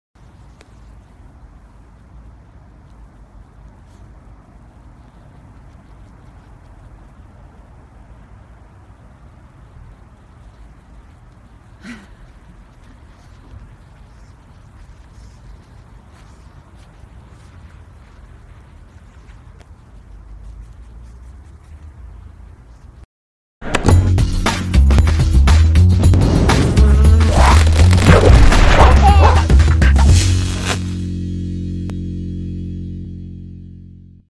Chihuahua plays dead when big dog approaches
When this Chihuahua sees Monica the 4-year-old Afghan Hound coming close it hilariously plays dead in order to avoid any confrontation. Too funny!